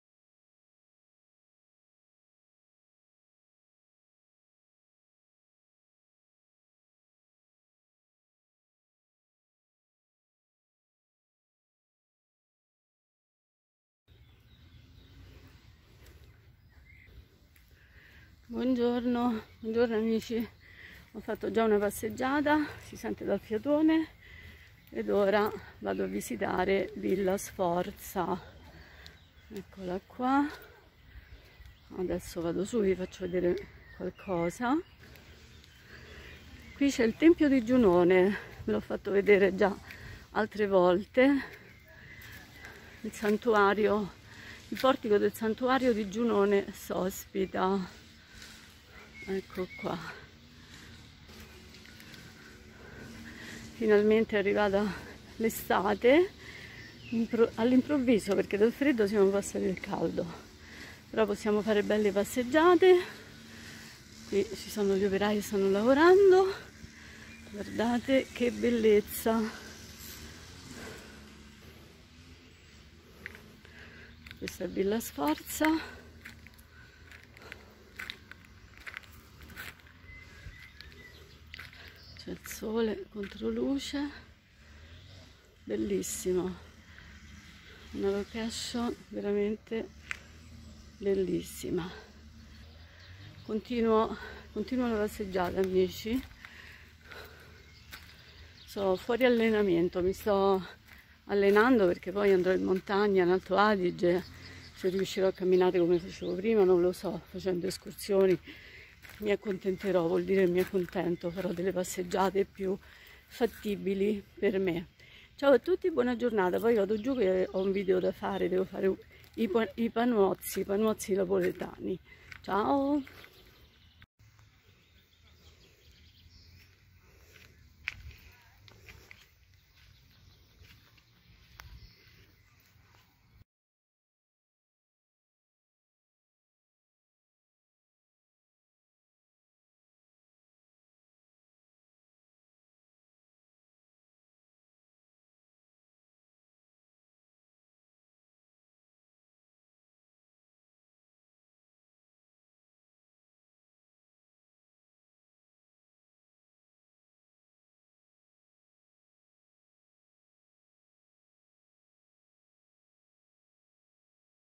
Vlog - passeggiata 15/06/2021
passeggiata mattutina
location Lanuvio (Roma) Castelli romani
Villa Sforza Cesarini
Tempio di Giunone Sospita